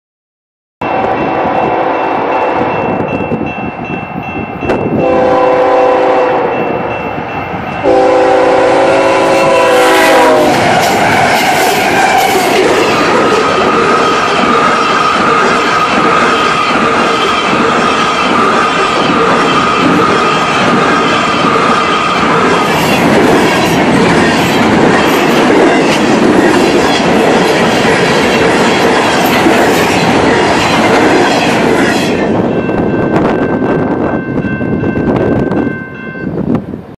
Short Freight Train on a Foggy Day
A short freight train crossing Side Road 25 in Puslinch, ON on a foggy December day